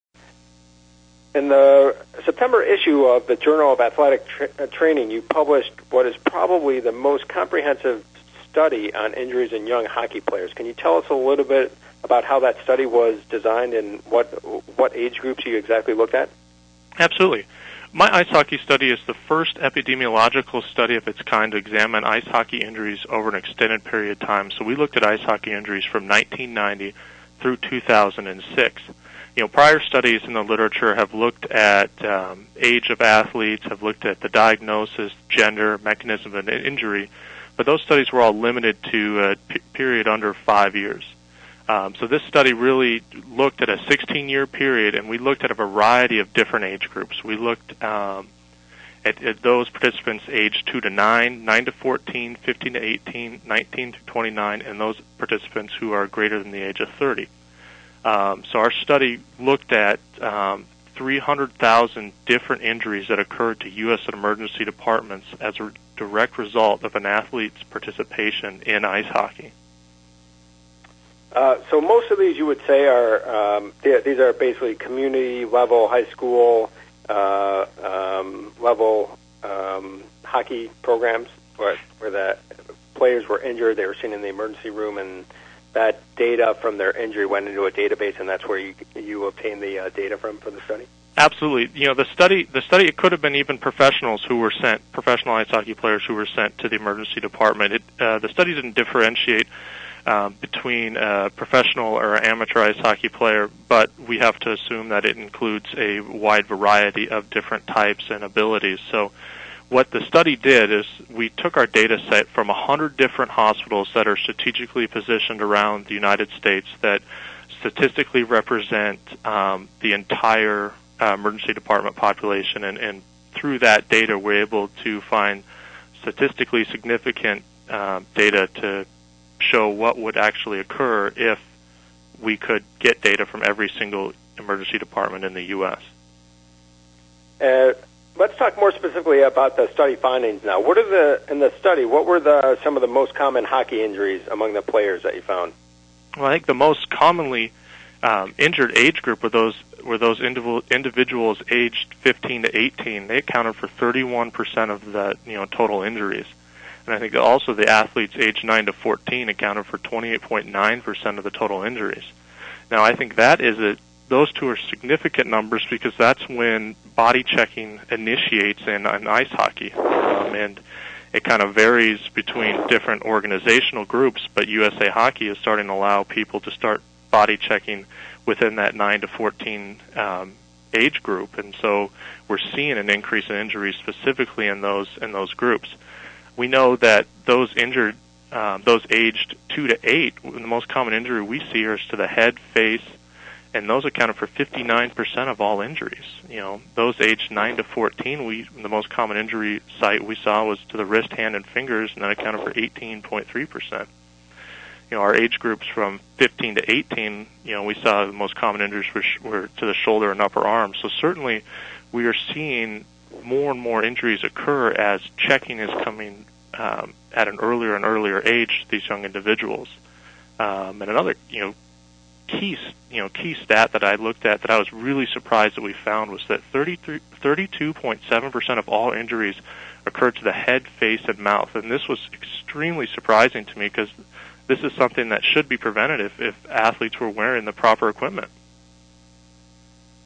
Serious Hockey Injuries Among Young Children Skyrocketing
Sports Injury Forum -Michael DeFranco MD | Guest
Jeff Deits
Athletic Trainer
Researcher, Hockey Injuries in Young Athletes